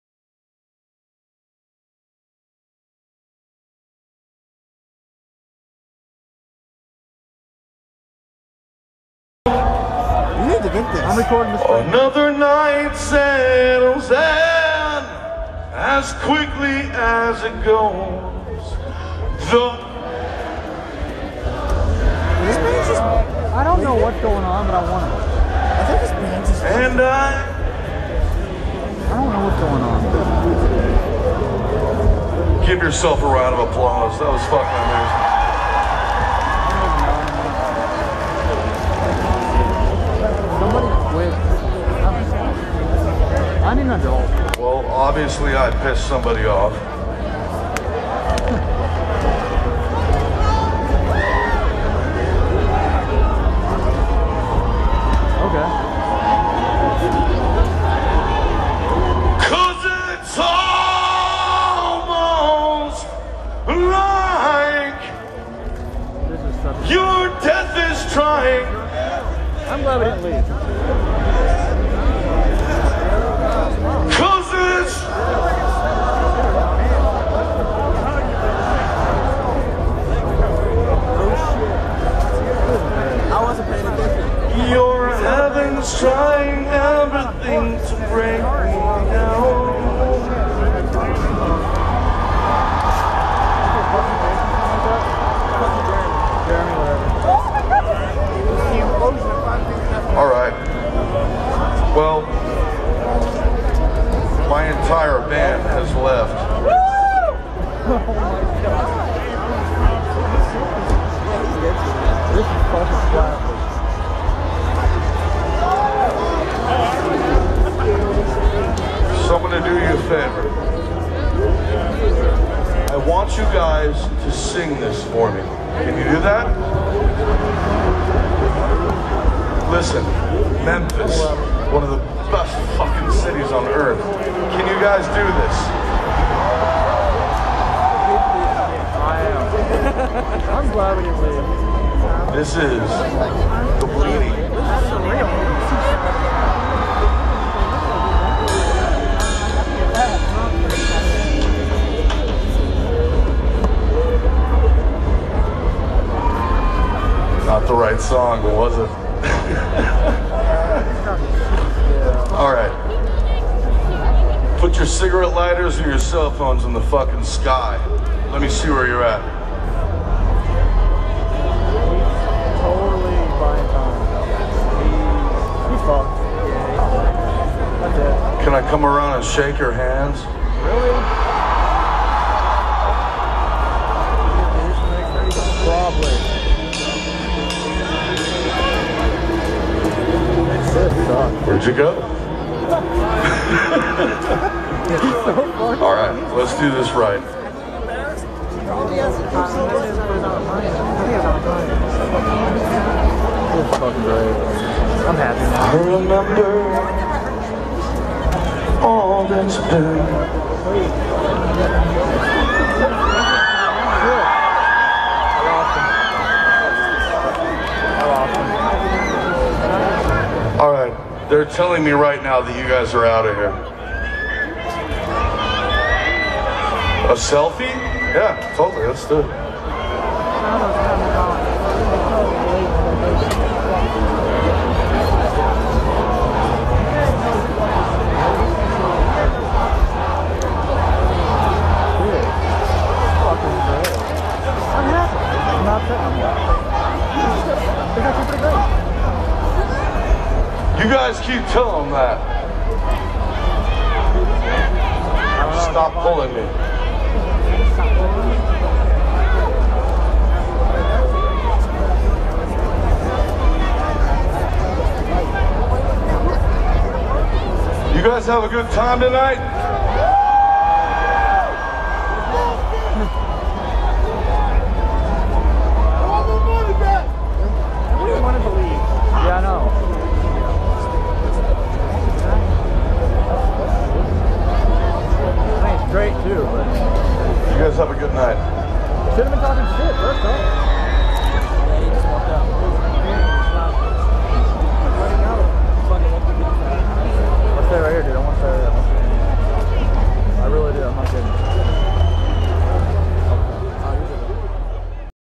Five Finger Death Punch Possible Break Up On Stage
So.. I didn't expect to see this going to the Memphis In May music festival.